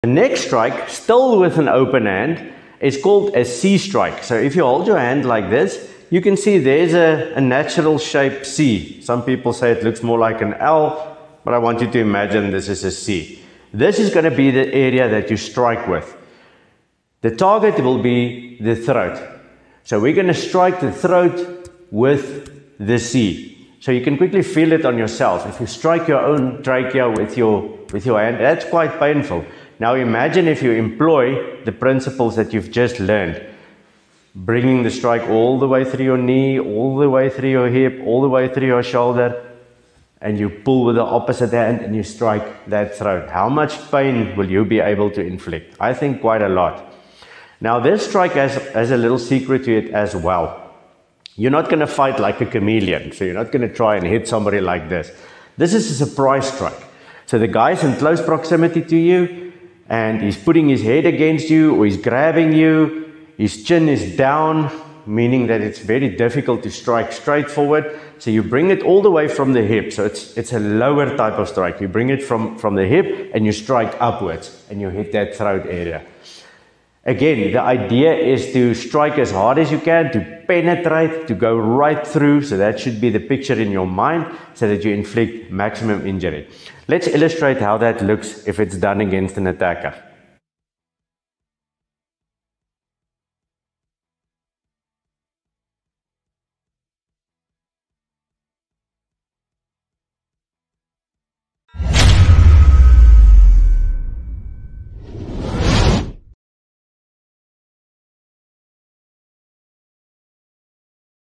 Unleashing the Power of C-Strike: How to Defend Yourself Against a Violent Criminal Attacker!
The C-Strike is a highly effective striking technique aimed at the throat of the attacker. It is very difficult to see it coming as it is applied at very close range  and from below the attackers field of view. On top of that, the throat is quite vulnerable and this strike can case some serious breathing impairment. This makes the C-Strike a great tool in your self defense arsenal.